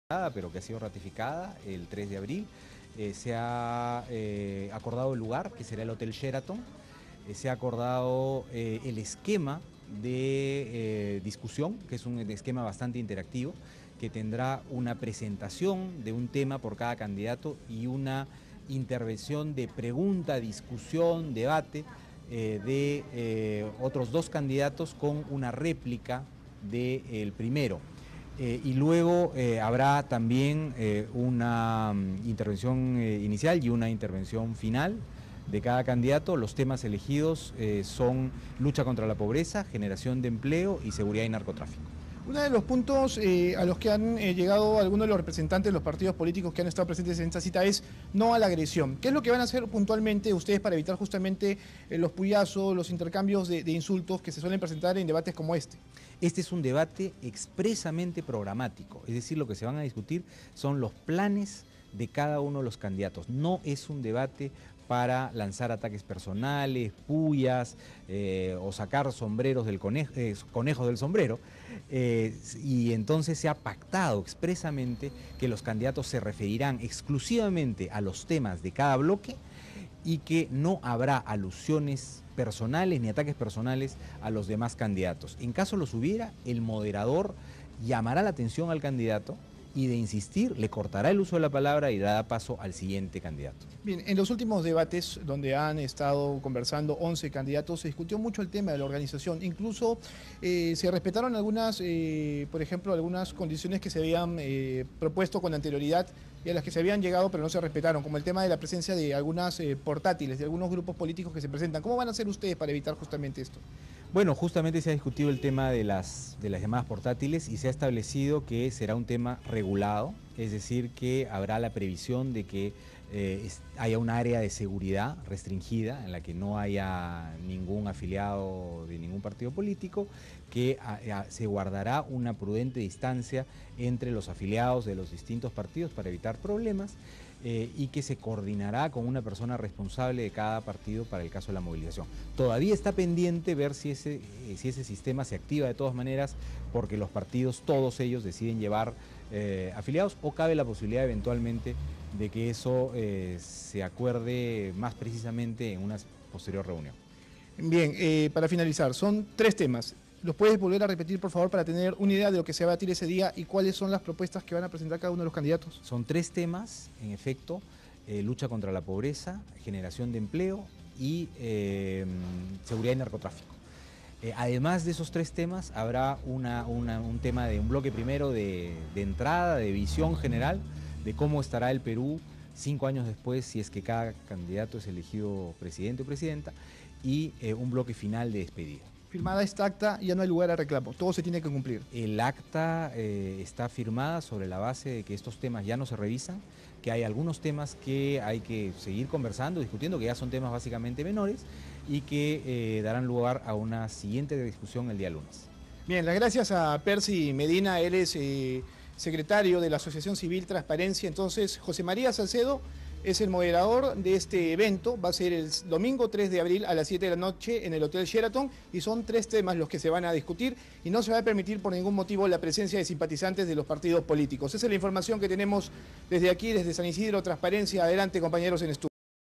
Debate presidencentre los cinco primeros serán en el Shearton - La Mula
El secretario general de Transparencia, Percy Medina, dio detalles del nuevo debate presidencial entre los cinco primeros candidatos a la república. Más informacion